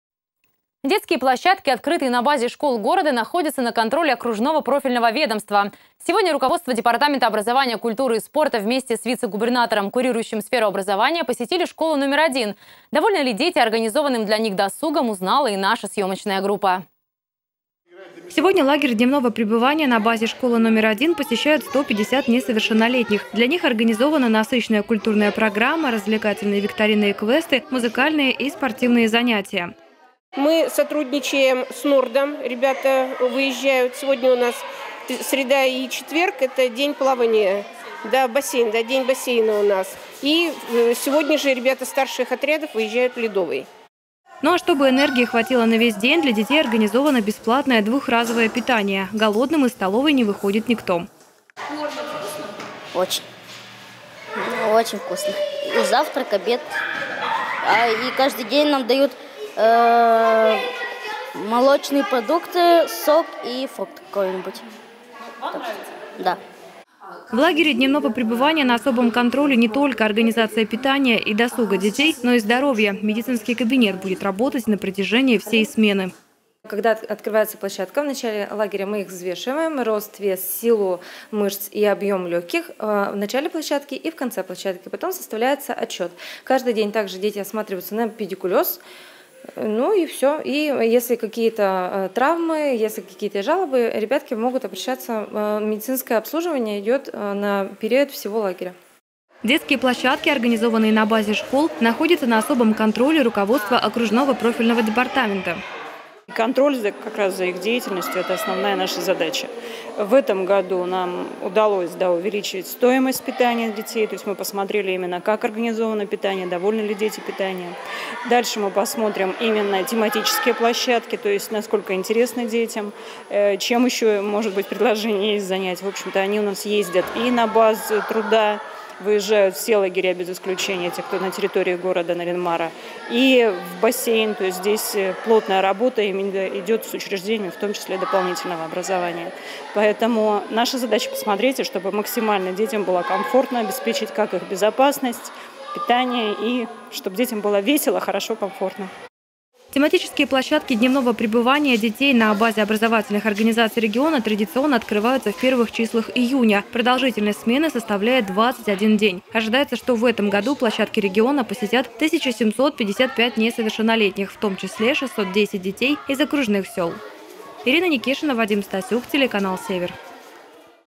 Пришкольные площадки
Детские площадки, открытые на базе школ города находятся на контроле окружного профильного ведомства. Сегодня руководство Департамента образования, культуры и спорта вместе с вице-губернатором, курирующим сферу образования, посетили школу №1. Довольны ли дети организованным для них досугом, узнала и наша съемочная группа.